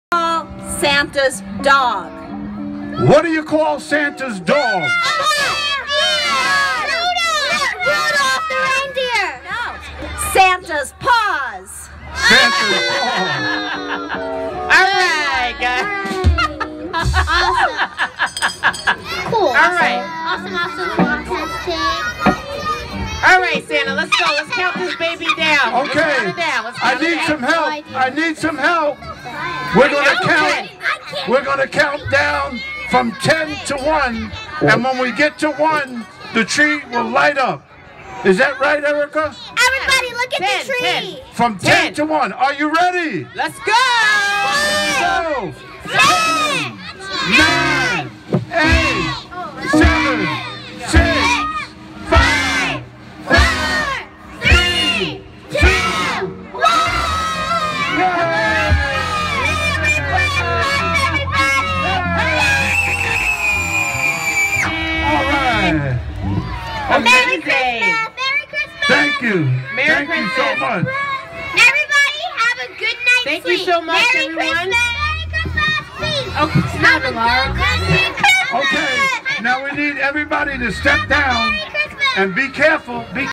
Roosevelt Island Holiday/Christmas Tree Lighting Ceremony Countdown
December 8 Roosevelt Island Tree Lighting Ceremony. Countdown To Tree Lighting